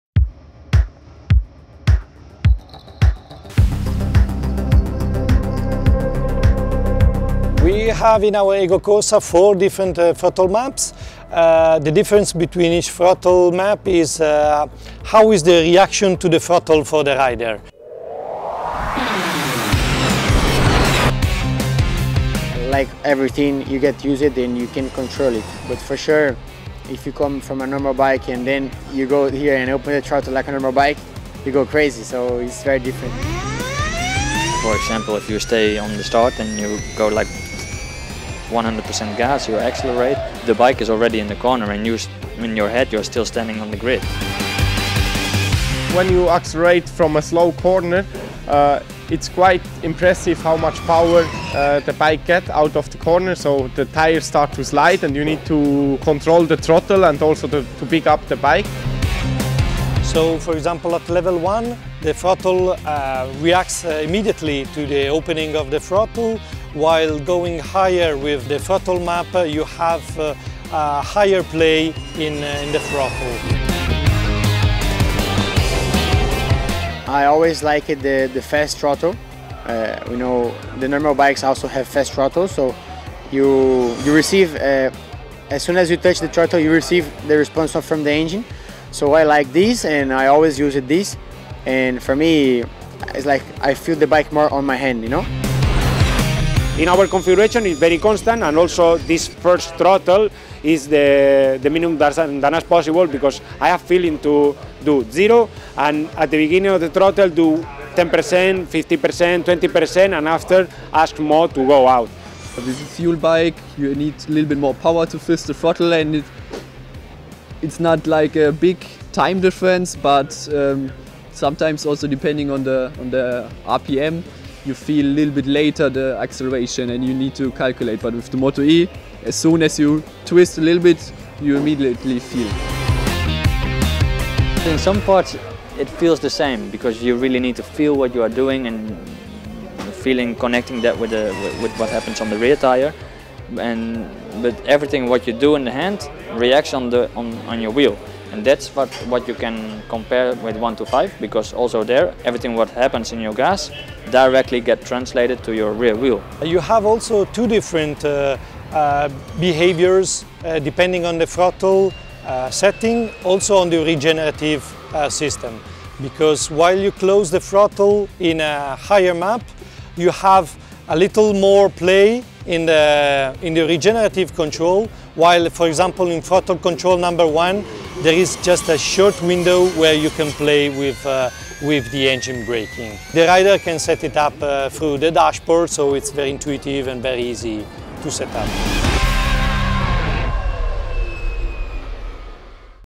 MotoE™ Tech Insights: Throttle configuration
With 100% torque at all times, getting your throttle configuration right is crucial! ⚠️

In MotoE, getting that right feeling is the key to success ⚡️